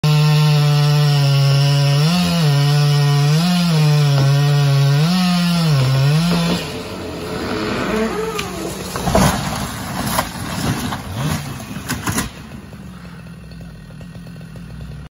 Fastest way to take a cabin down
Owner wanted it gone soooo we solved his problem lol